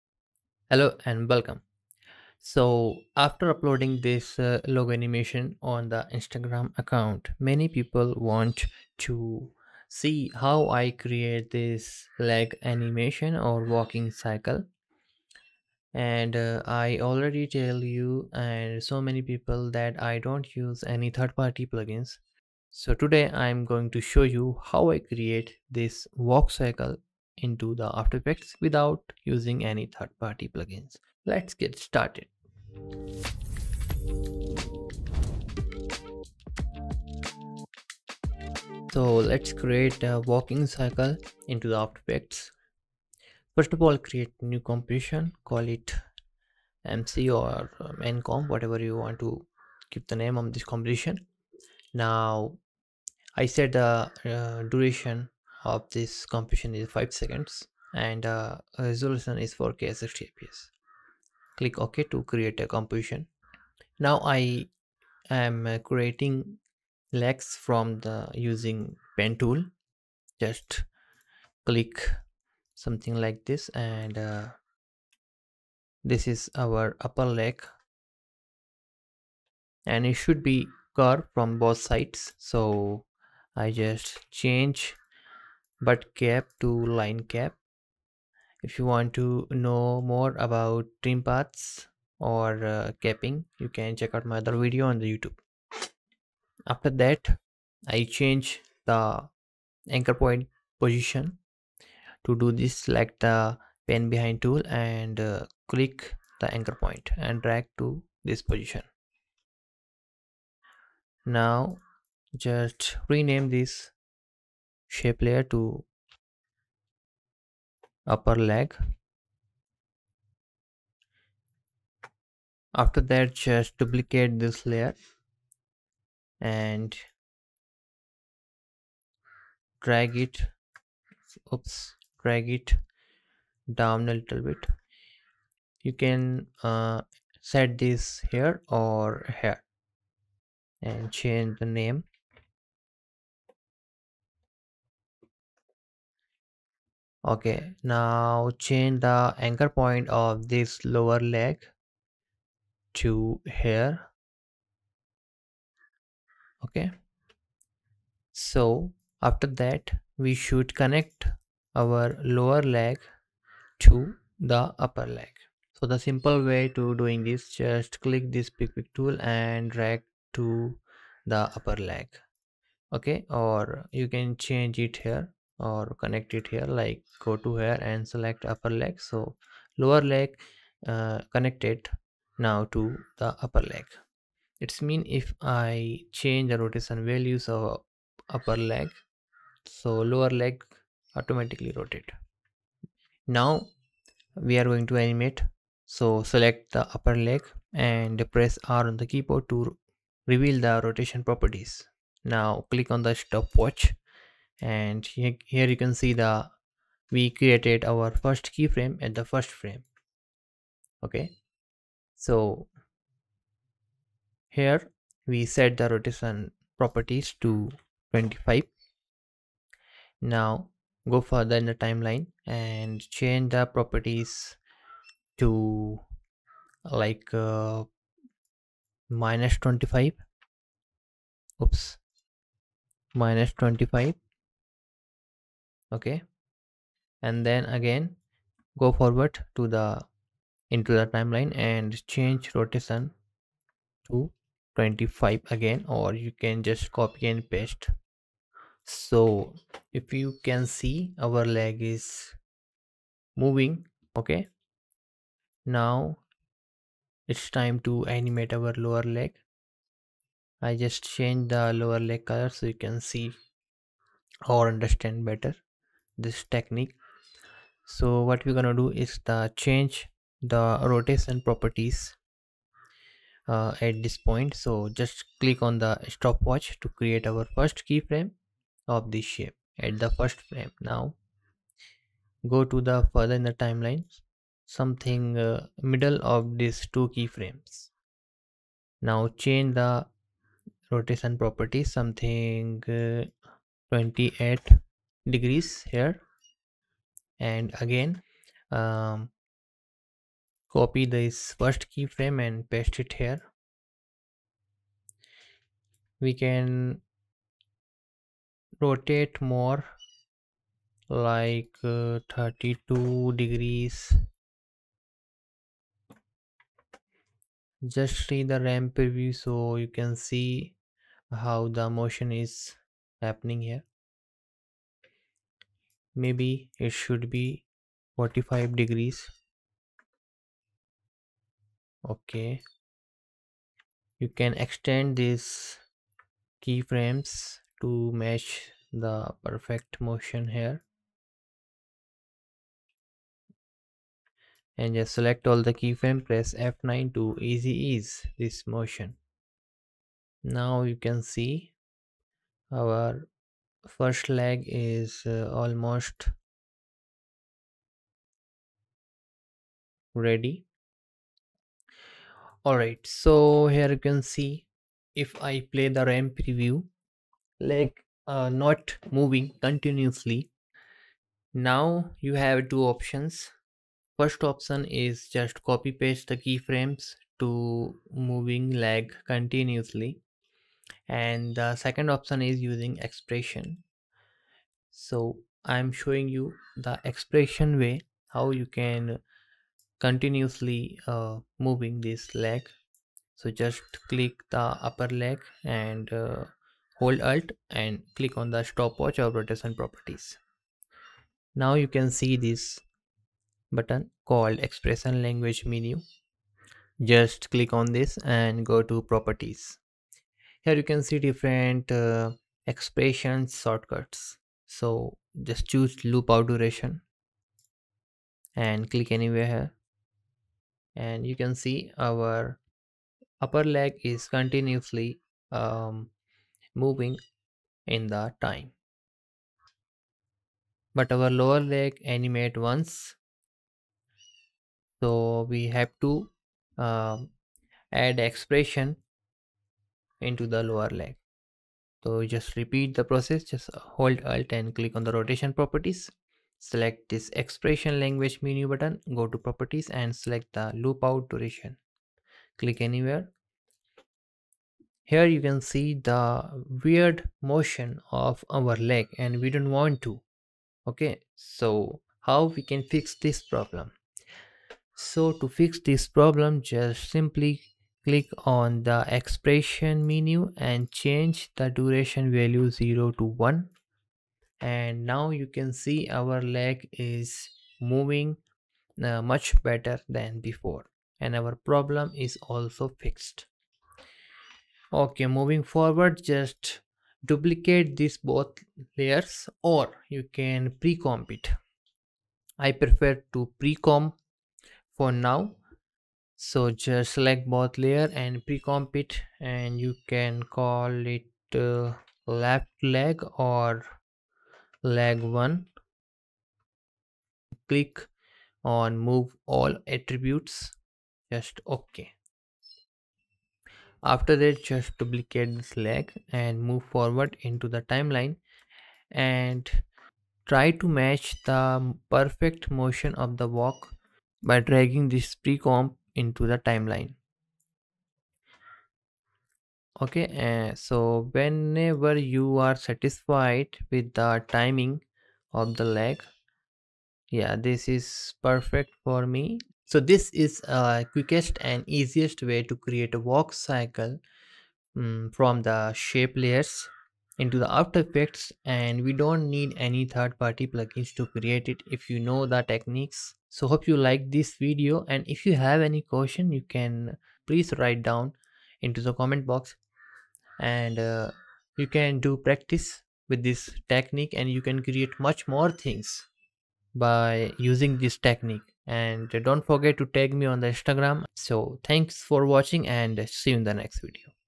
walk cycle - After effects Tutorial by sheikh sohel
the walk cycle sounds like very difficult thing in the motion graphics industry but if you ask me, walk cycle is very easy if you understand the basics of walk principles and you can create a wonderful walk cycle into the After Effects without using any third party plugins and I'm showing you in this video some basic techniques which can help you to create your first walk cycle using shape layers into the After Effects and you don't need any third party plugins to create your first walk cycle animation.

I am also showing you some expressions which we will learn in this tutorial lt can help you to improve your workflow into the After Effects and these expressions very useful especially when you try to implement into the walk cycle.